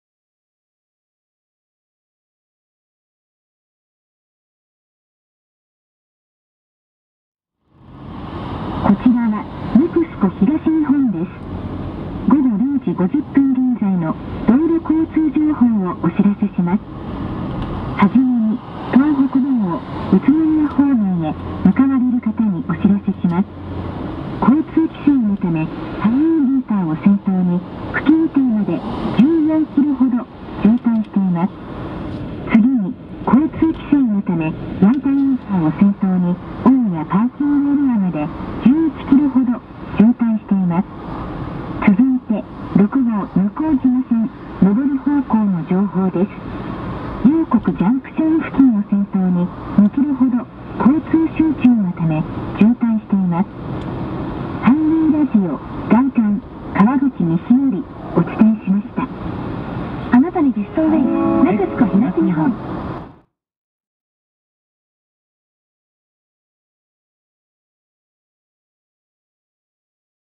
え？交通規制で東北道大渋滞？ 外環・川口西
詳しい状況はわからないのですが、東北道で交通規制により10キロ超えの渋滞が二か所発生していました。